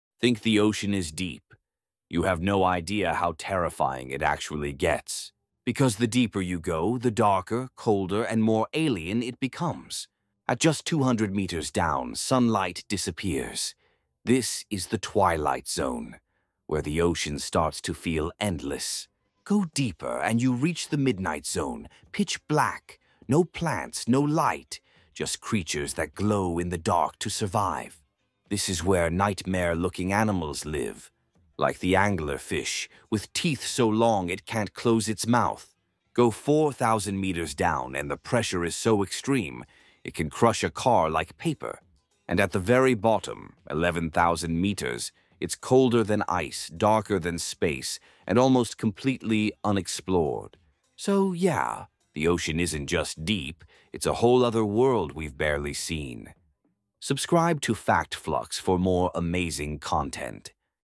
The Ocean Is WAY Scarier Than You ThinK
Explore how deep and terrifying the ocean really is — from the Twilight Zone to the Mariana Trench. Dark waters, alien creatures, crushing pressure… and a world humans barely understand. 🌊
Perfect for anyone who loves science, mystery, or the unknown.